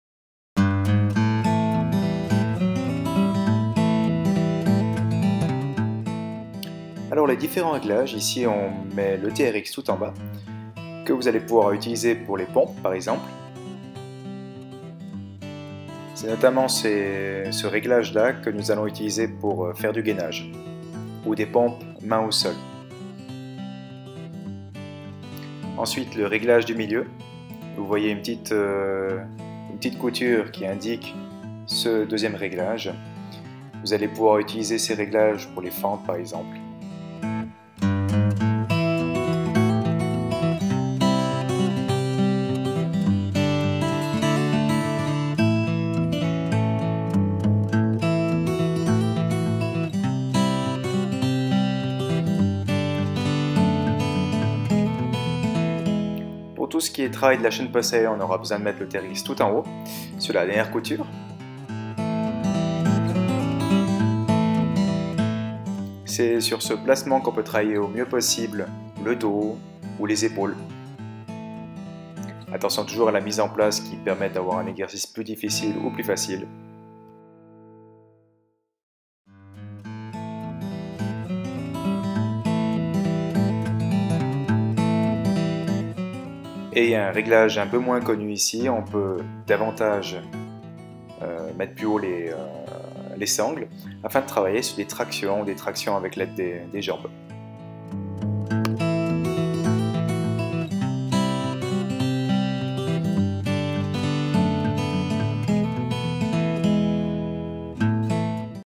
A quelle hauteur régler ses sangles de suspension?
Optimisez votre entrainement en utilisant correctement votre matériel.